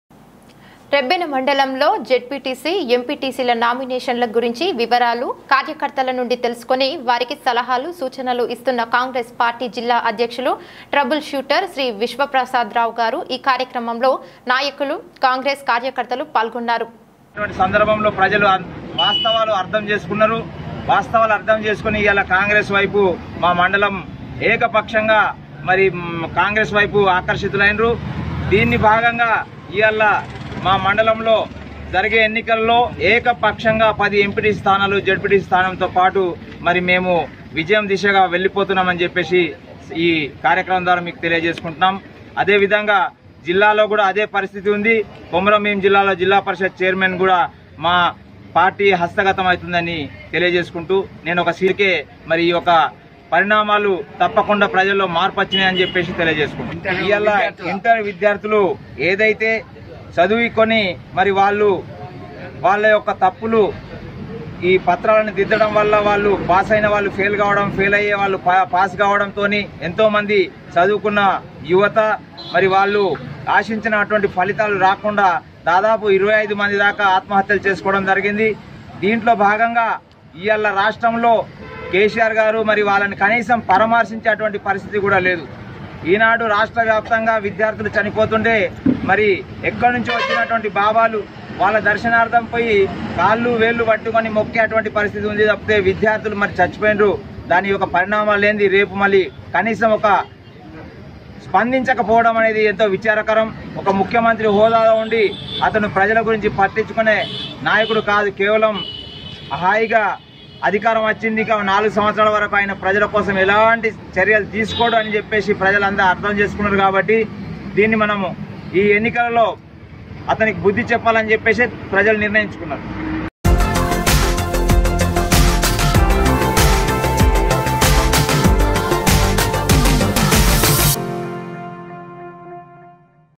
ZPTC , MPTC ఈ కార్యక్రమంలో నాయకులు , కాంగ్రేస్ కార్యకర్తలు పాల్గోన్నారు -Ev6 Tv
రెబ్బెన మండలములో  ZPTC , MPTC ల నామినేషన్ల గురించి వివరాలు  , కార్యకర్తల నుండి తేలుసుకొని  , వారికి సలహాలు , సూచనలు ఇస్తున్న కాంగ్రేస్ పార్టీ జిల్లా అద్యక్షులు ట్రబుల్ షూటర్ శ్రీ విశ్వప్రసాద్ రావు గారు ,  ఈ కార్యక్రమంలో నాయకులు , కాంగ్రేస్ కార్యకర్తలు పాల్గోన్నారు.